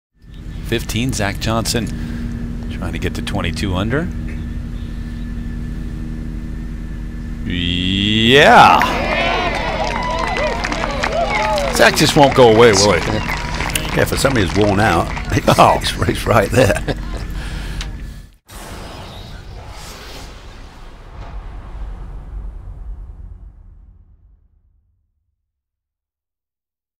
Zach Johnson makes 26-foot birdie putt at Humana Challenge
In the final round of the 2014 Humana Challenge, Zach Johnson sinks a 26-foot birdie putt on the par-3 15th hole.

For complete coverage of the Humana Challenge .